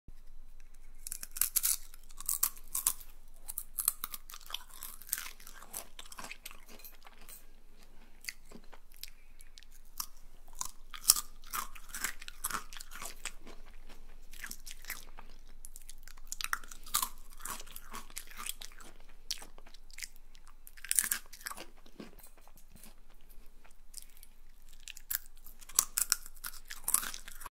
ASMR chewing and biting apple chips 씹고 깨무는 사과칩
In this video, you will hear the mesmerizing sounds associated with eating and chewing crunchy apple chips. ASMR videos are perfect for relaxing, relieving stress, and immersing yourself in a world of soothing sounds. Every squeeze and chew will be transmitted to you with maximum clarity so that you can enjoy every moment. Enjoy the crunch and full flavor of apple chips with us. Write in the comments what you think of the video and which ASMR sounds you like the most!